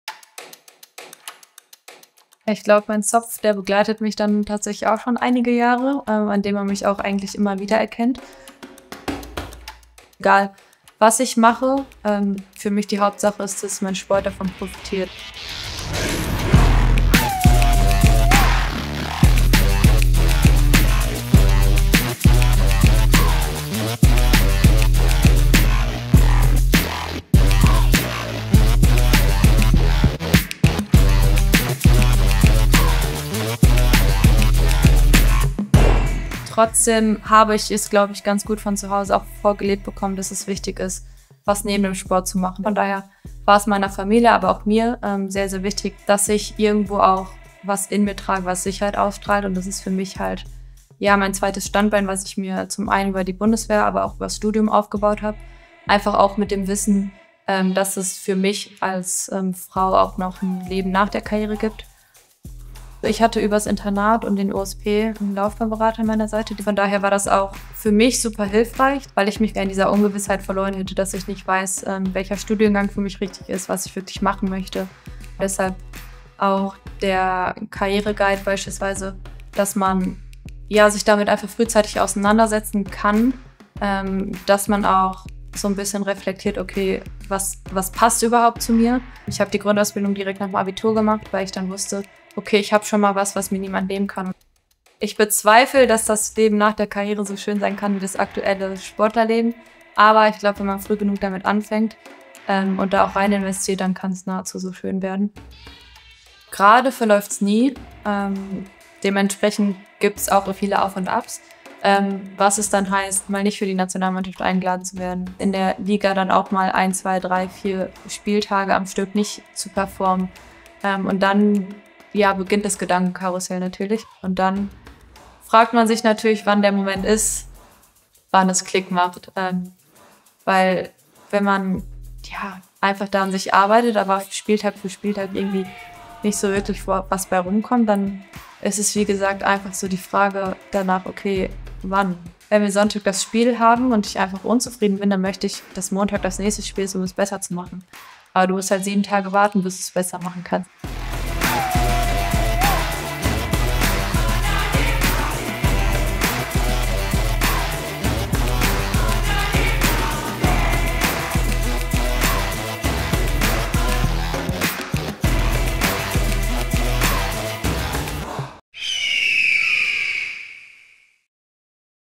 "Gerade verläuft es nie" I Sophia Kleinherne über ihren Karriereplan
In der ersten Folge unseres neuen Video-Formats "Indeed Karrieretalk" spricht Verteidigerin Sophia Kleinherne darüber, wie sie sich über Bundeswehr und Studium schon früh auf ein Leben nach der Karriere im Profifußball vorbereitet hat, was ihr bei der Orientierung geholfen hat und welche besonderen Herausforderungen das Sportlerinnen-Dasein mit sich bringt.

Wenn ihr selbst Lust auf ein neues berufliches Abenteuer im Herzen von Europa habt, schaut mal hier vorbei und werdet Teil des Eintracht-Teams